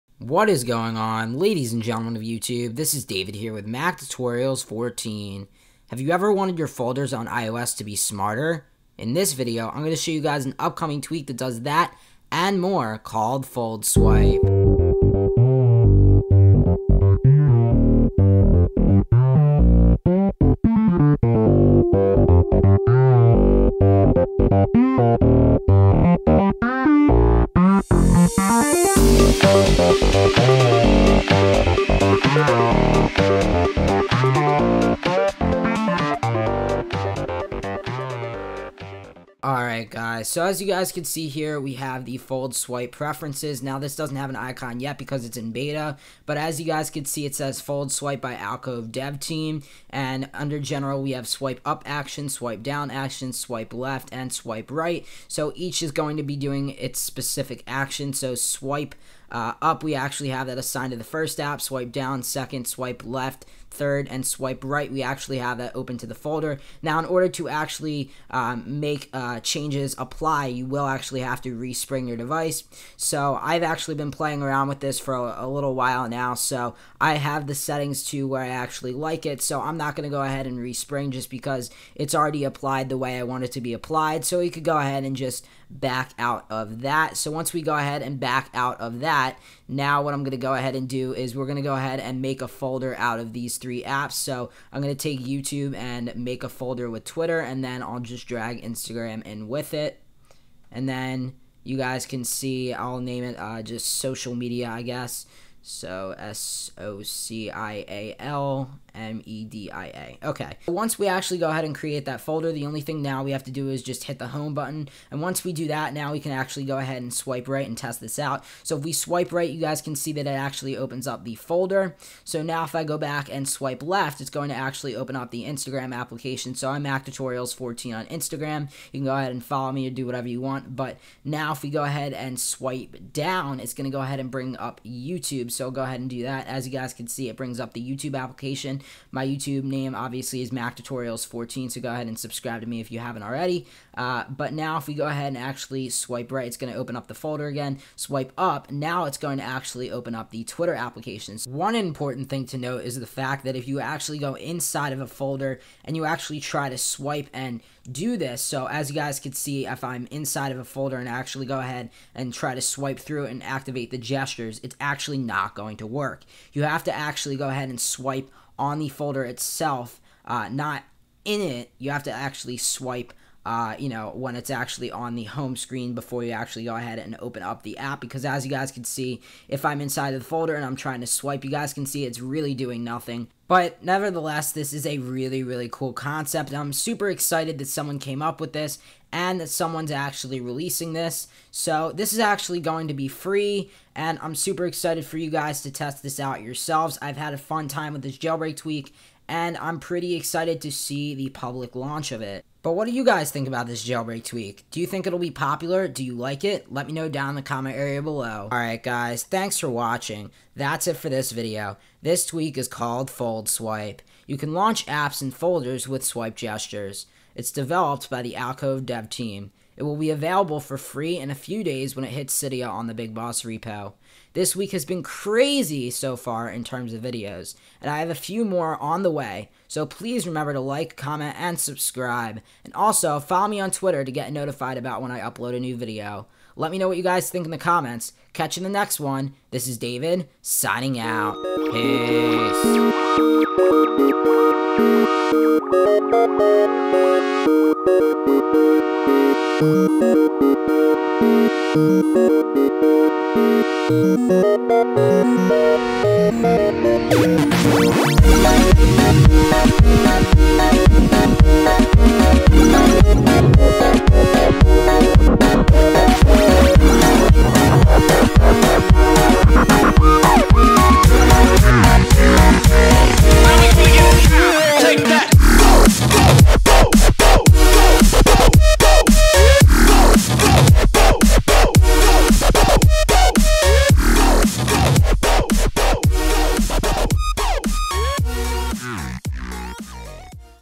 JAILBREAK TWEAK PREVIEW:FOLDSWIPE
Tweak: FoldSwipe
Description: an upcoming tweak that lets you launch apps in folders with swipe gestures.
Developer: AlcoveDevTeam
Repo:It will be on BigBoss!
Price: It will be free!

Let me know what you guys think about this video in the comments! Should I make more jailbreak tweak reviews? Thank you to the guys at @AlcoveDevTeam for making this amazing jailbreak tweak and giving me an early copy!